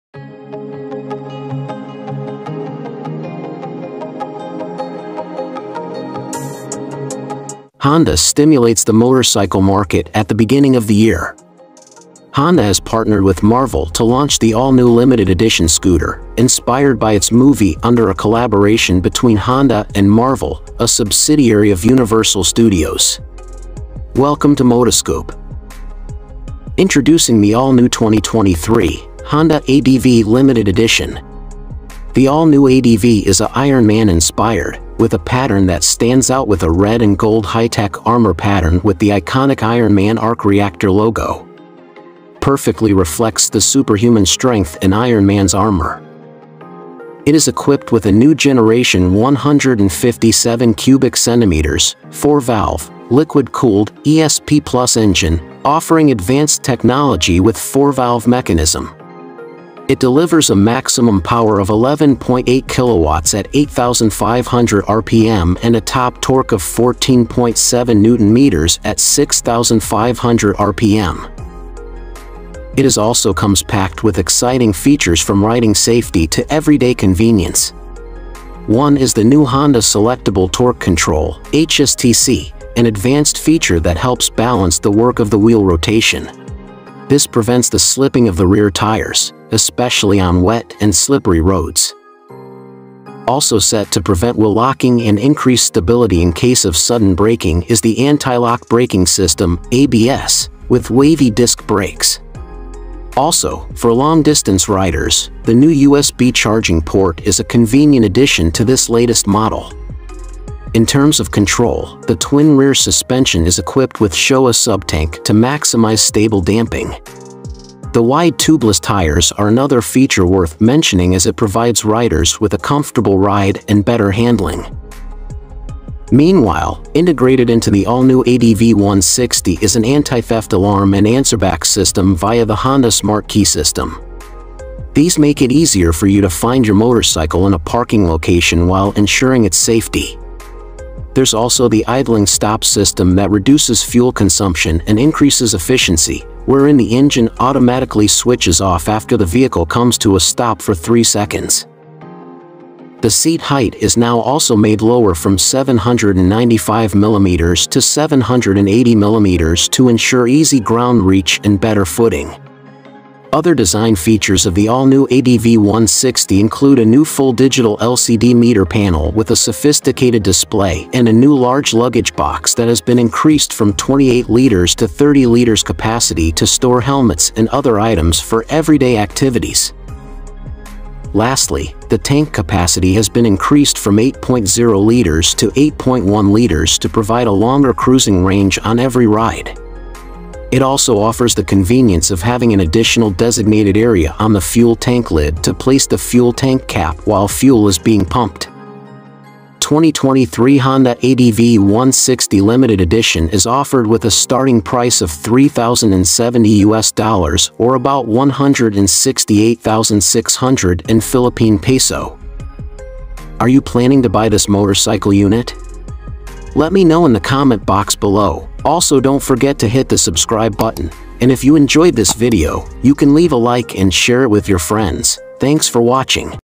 2023 Honda Limited Edition 160cc Adventure Scooter Launched With New Graphics – ADV Walkaround
2023 Honda Limited Edition 160cc Adventure Scooter Launched With New Graphics – ADV Walkaround